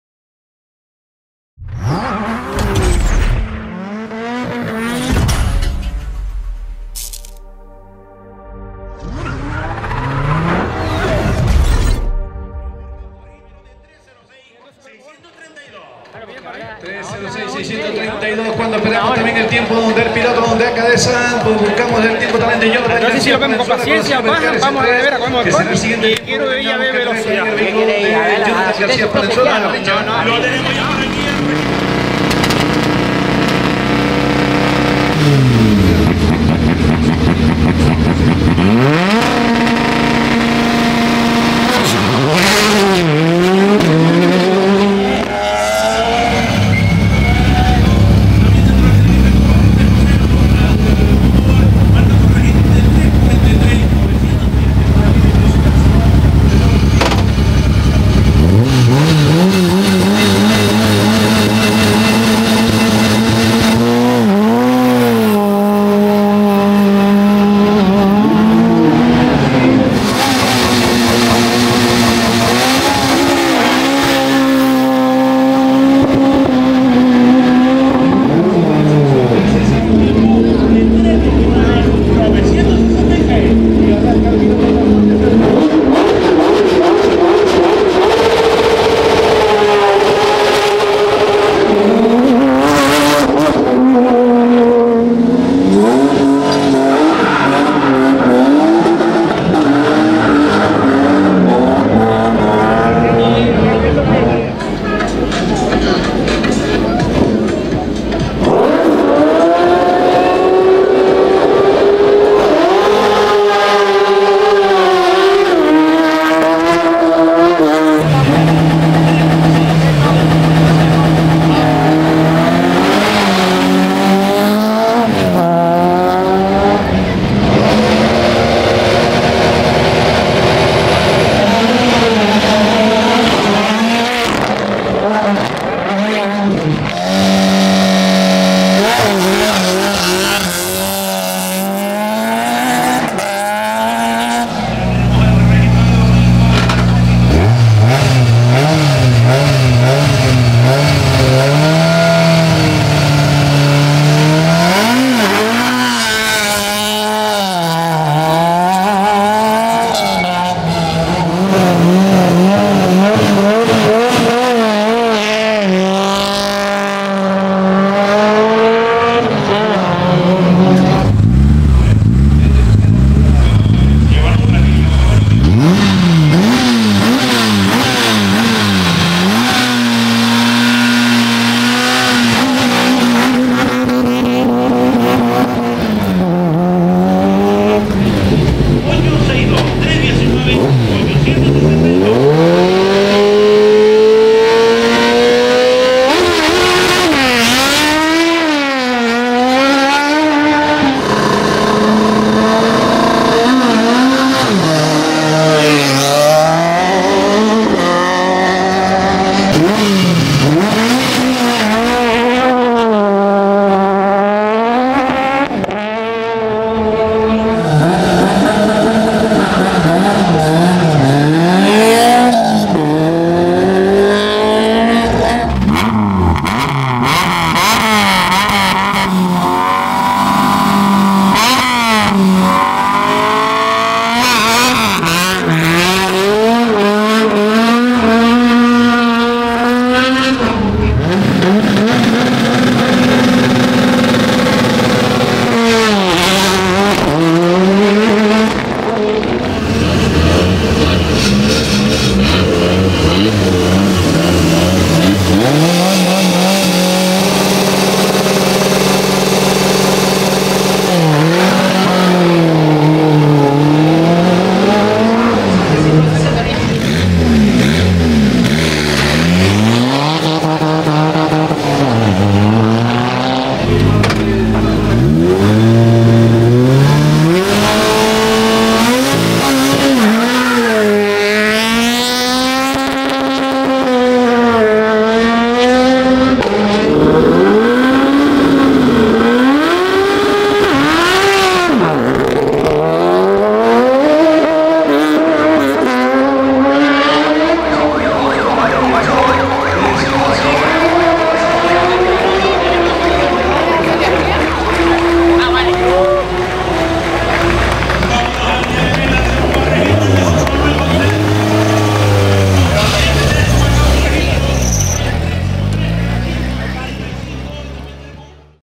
La salida de la Subida a La Guancha 2023 - LAUNCH CONTROL, REVS & HIGHLIGHTS
La cuenta atrás más larga de tu vida, así es el minuto 00:00 de una prueba de montaña.
Nervios, un temple de santo con precisión de cirujano en el embrague, gas a fondo, y en el 3, 2, 1... si parpadeas te lo pierdes.